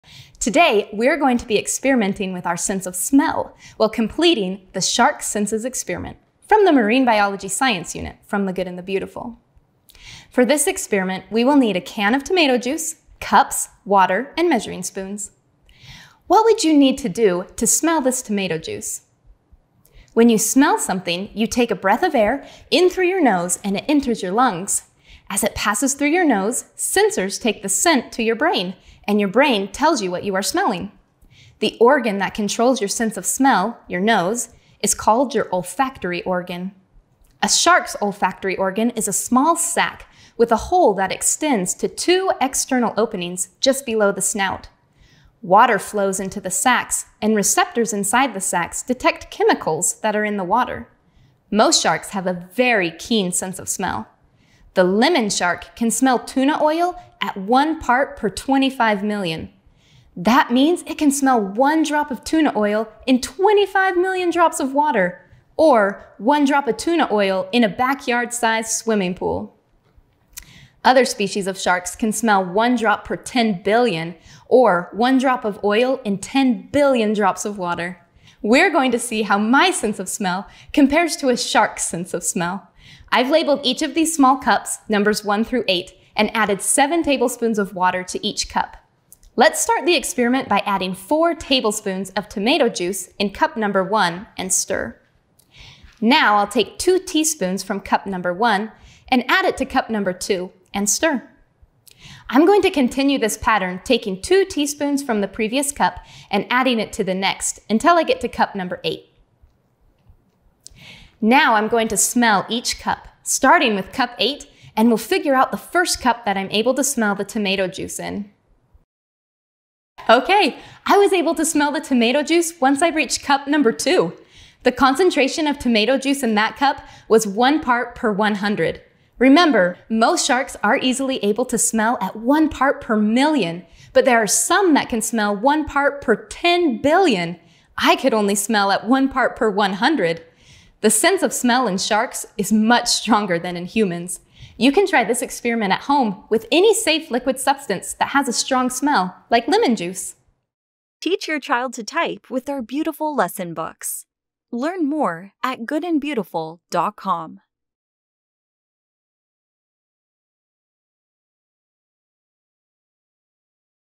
How Good is a Shark's Sense of Smell Experiment | Marine Biology | The Good & the Beautiful
How good is a shark's sense of smell? How good is your sense of smell? Compare the two in this fun and easy science experiment for kids! From Lesson 10 of the Marine Biology unit by The Good and the Beautiful.

(0:00) Introduction
(0:10) Preparation
(1:29) Experiment
(1:59) Conclusion

Materials needed:
Can of tomato juice
8 glasses
Water
Measuring spoons

🦈 Look inside the Marine Biology unit: youtu.be/VcYTVAFtmXY